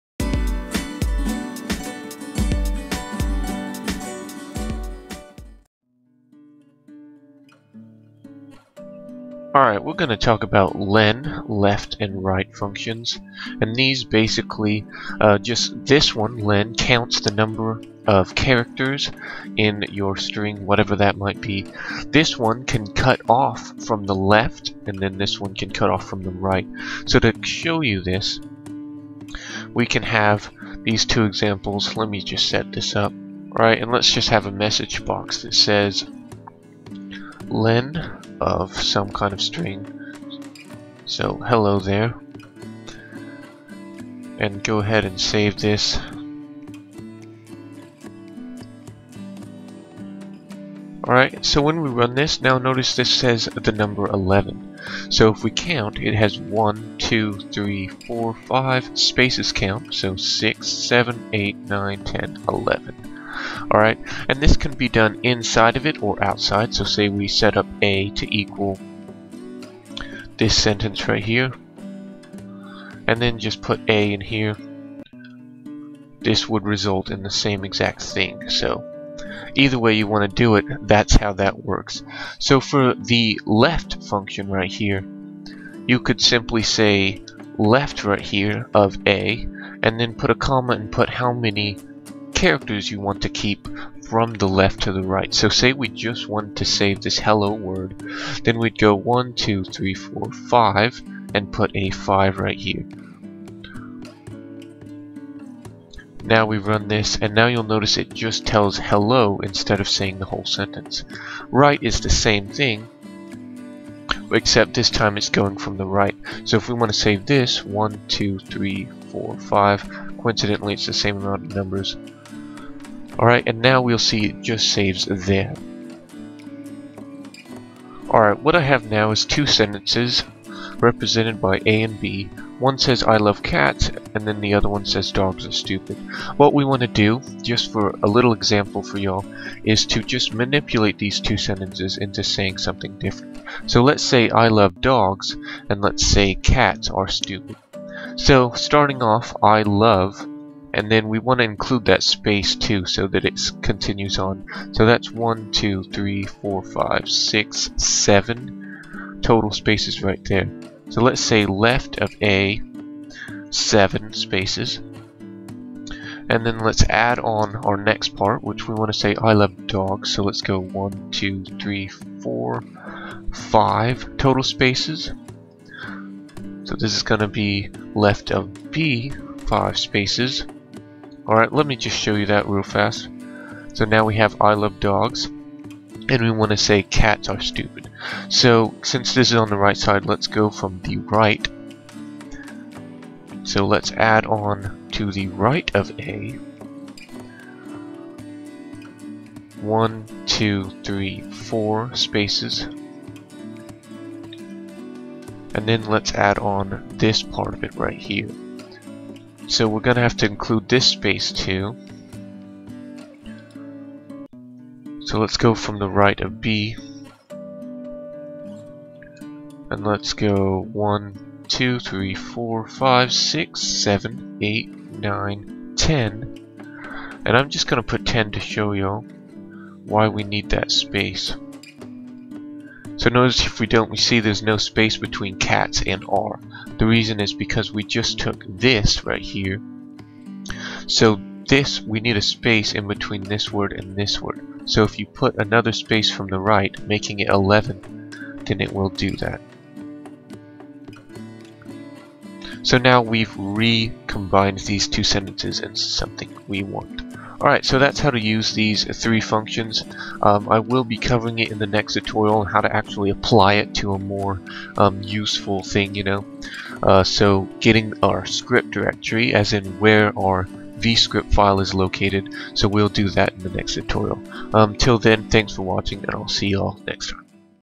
VBScript Basics, Part 9 | Len - Left - Right
VBScripting (.vbs) Basic tutorial on how to use the len, left, and right function. Len which converts your string into a numeric value, and Left which allows you to save characters of a string from left too right, as well as right which goes the other way.

Option Explicit
Dim a, b

a= "I love cats"
b= "dogs are stupid"

msgbox Left(a,7) & Left(b,5) & Right(a,4) & Right(b,11)

msgbox "there is " & Len(a) + Len(b) & " characters in this string."

Know the Basics:
Len(string) = # of characters in a string.
Left(string, #) = keep characters from left too right.
Right(string, #) = keep characters from right too left.